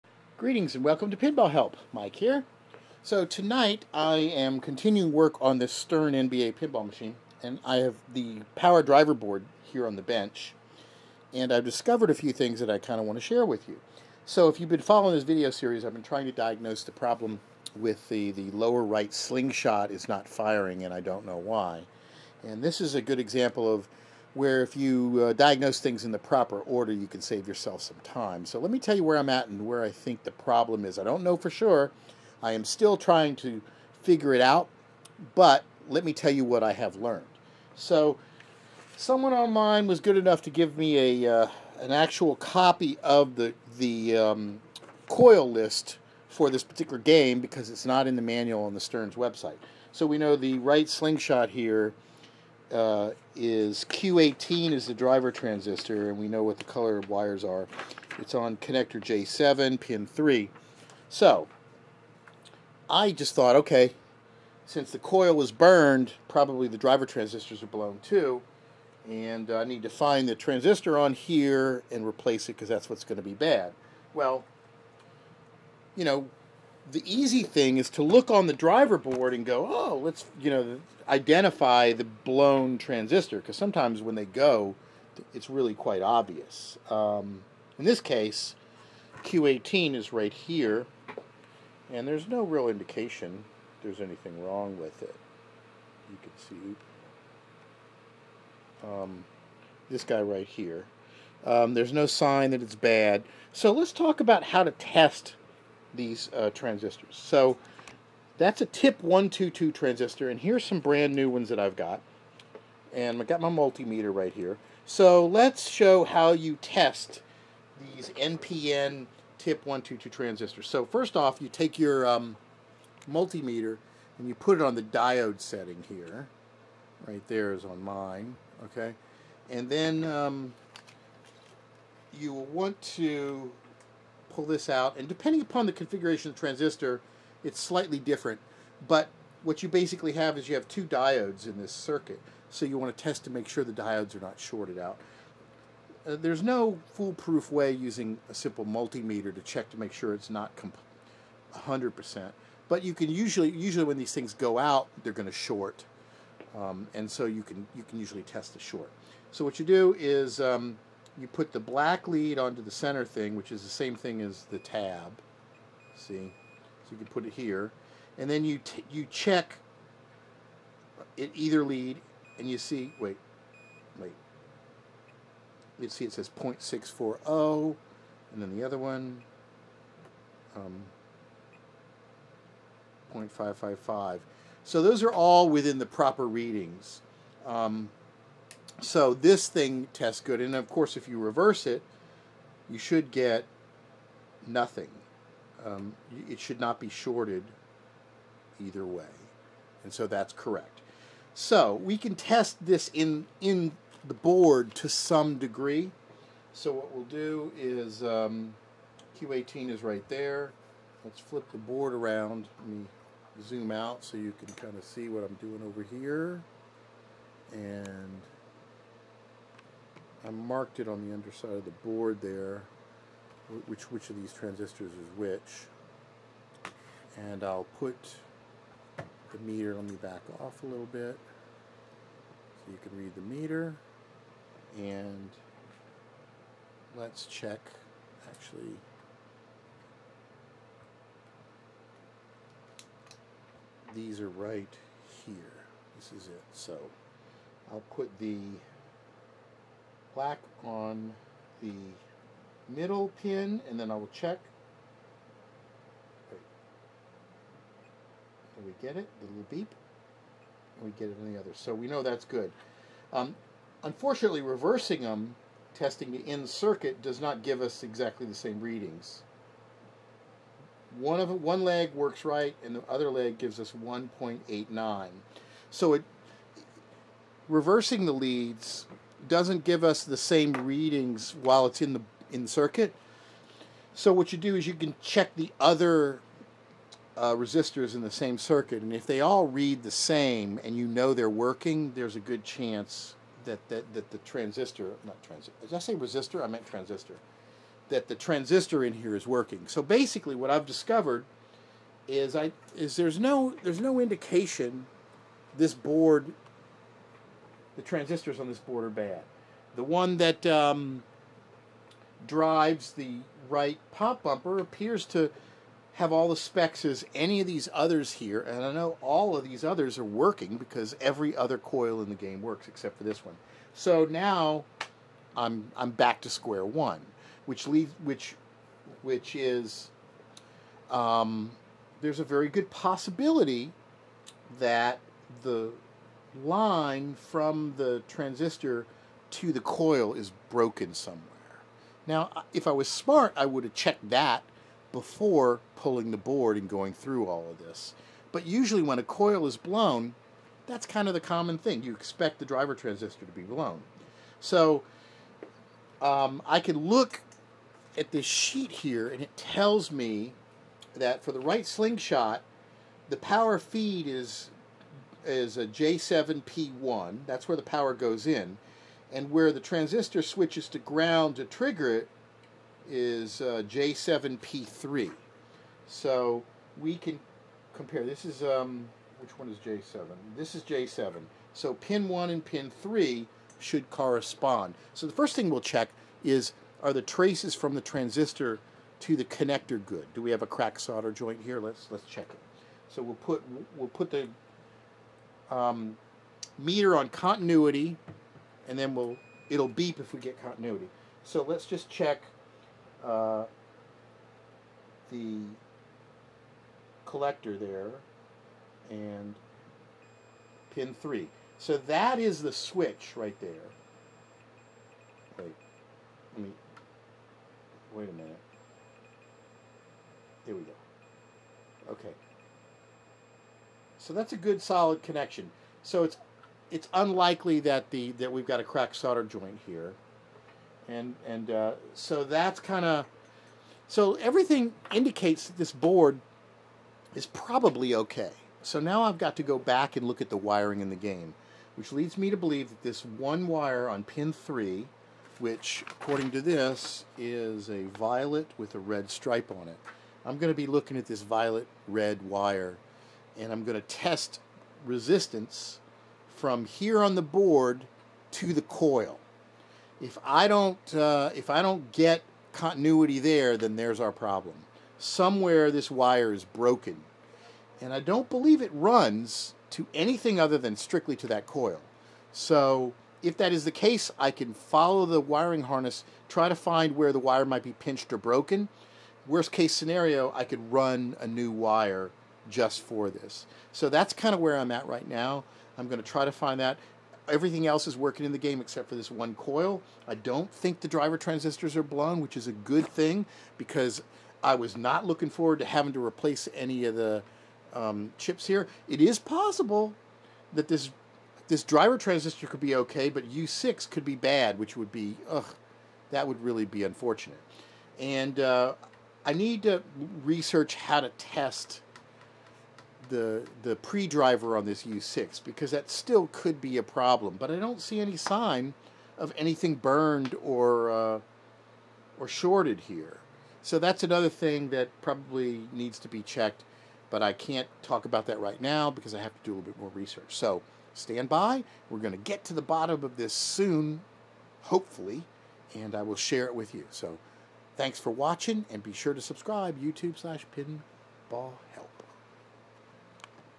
2009 Stern "NBA" Pinball p6 - Diagnosing coil issues (right slingshot) - PinballHelp.com
Work continues trying to diagnose why the lower right slingshot coil is not working.  In this video I'm bench testing the Stern S.A.M.  power/driver board and begin to check driver transistors.  There's another part to this story in the next video you want to see...

A very rare 2008 Stern "NBA" pinball machine.  This game was made right when the recession hit the country and it's a very low production game very few people have had a chance to play.  I'm excited to take a look at the condition and see what's involved in making it playable.